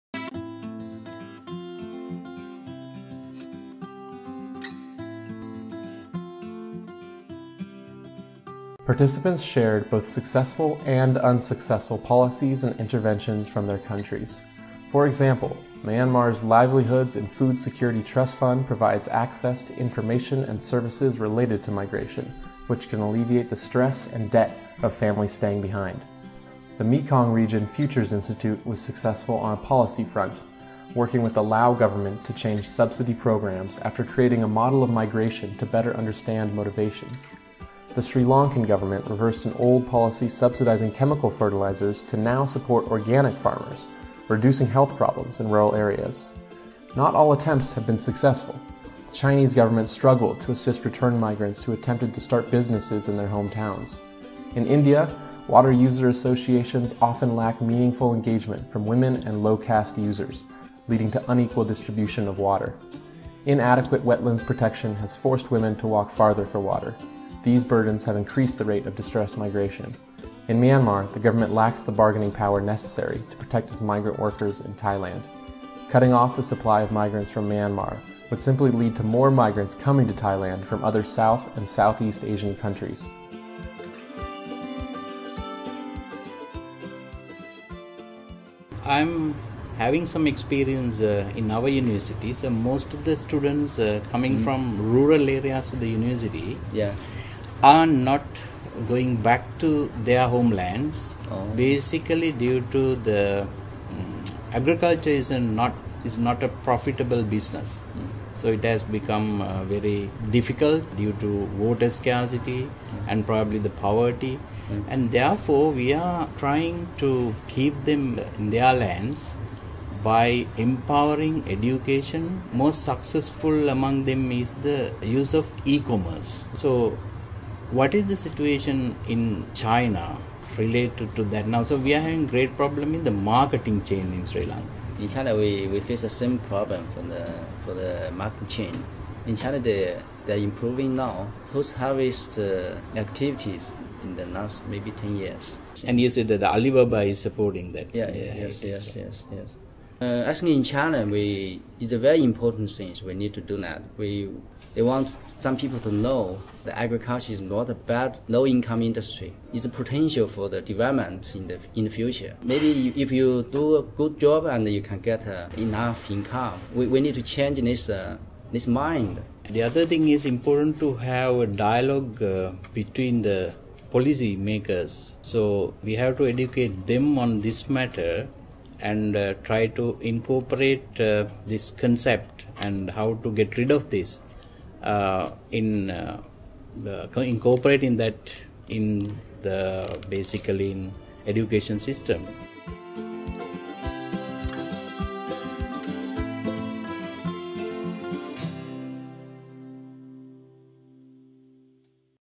MARIS Video Series (Pt. 3): Analyzing existing policies and interventions
Part three of the Migration, Agriculture and Resilience Initiatives for Sustainability (MARIS) video series. Learn more about the ongoing work of MARIS